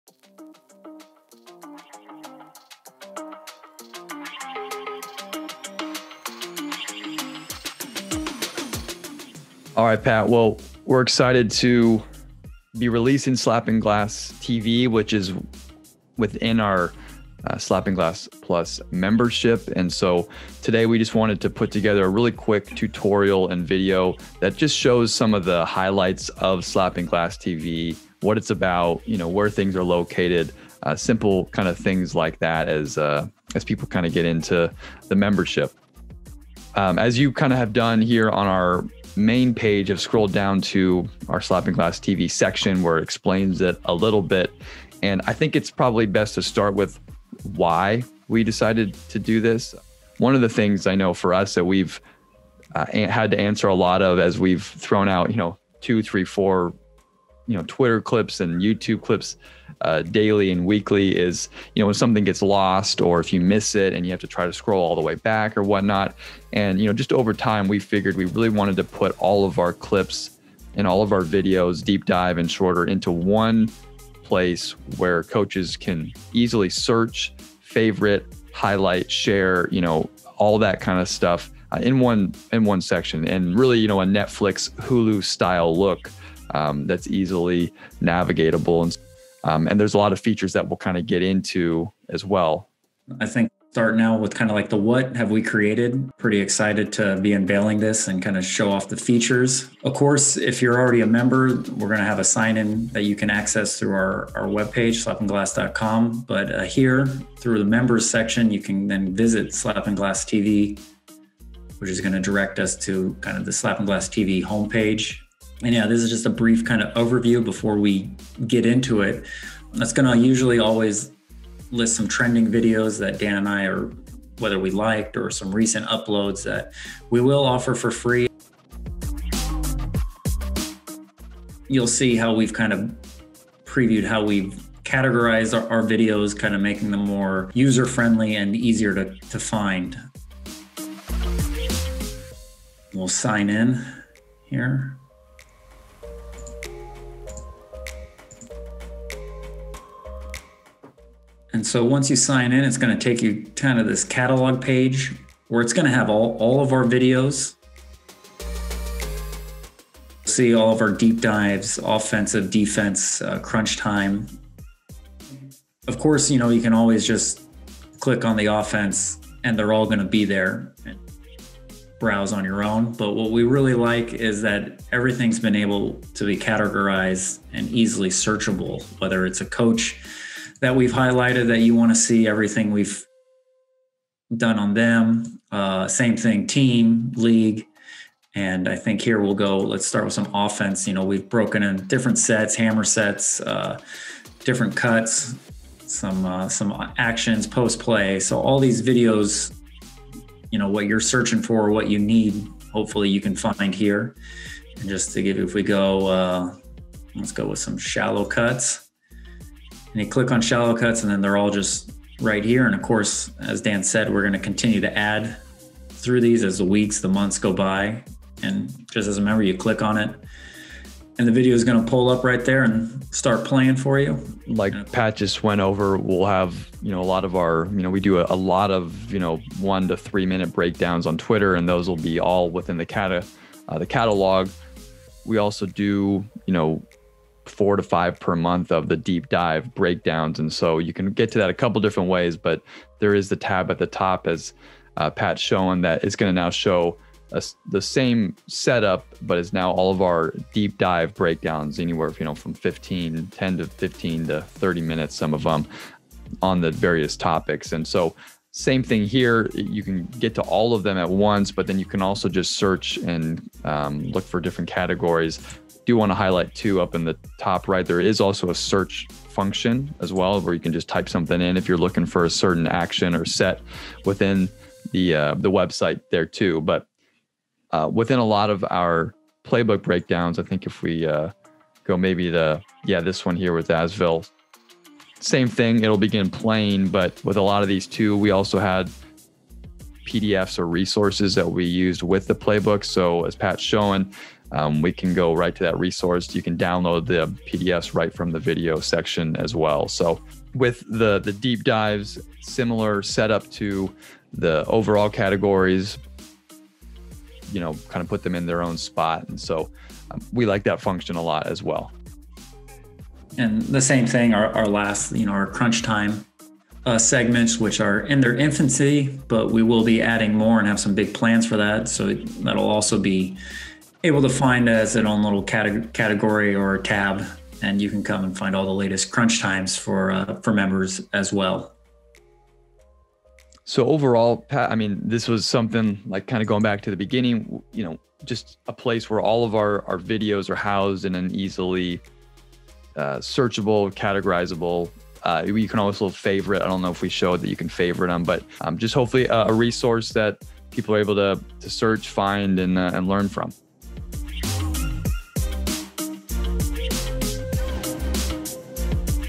Slappin' Glass TV Tutorial
Announcing Slappin' Glass TV, an exclusive component to our Slappin' Glass Plus membership. Members will have access to all our short length breakdowns, deep dive breakdowns & Crunch Time sessions through Slappin' Glass TV. With each video categorized to provide ease of access and the ability to search for any topic, tactic, team, coach or set you may need. For more on Slappin' Glass Plus and how to begin receiving Slappin' Glass TV visit our